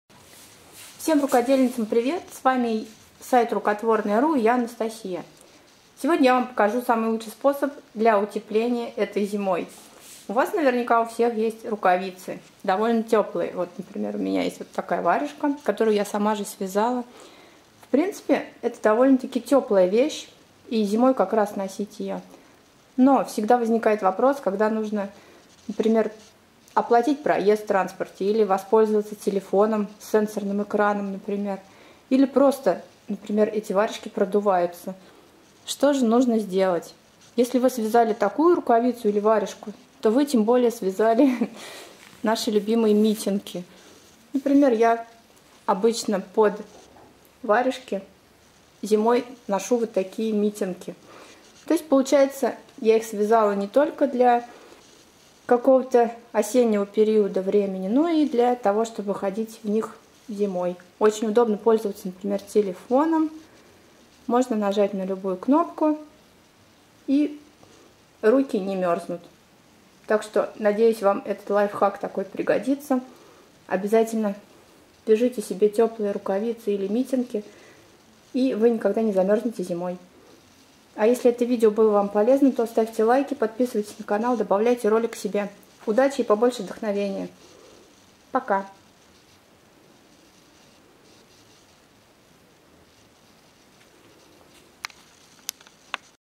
Лайфхак для рукодельниц - как согреть руки зимой?
В видео делюсь опытом – как сделать так, чтобы руки не мерзли зимой. Этот способ идеален для тех, кто вяжет. Ведь если хочешь сделать что-то безупречно – сделай сама :)

Чуть больше года назад я стала обладательницей телефона с сенсорным экраном (о моем предыдущем телефоне уже был пост ), зимой пользоваться телефоном стало гораздо сложнее. Специальные перчатки я не покупала, ношу собственноручно выполненные варежки. А мой способ применения вязаных вещей в условиях морозов – в этом видео.
И отдельно хочу еще раз напомнить о том, что нужно беречь свои руки в холодное время года и не выходить из дома зимой без утепления!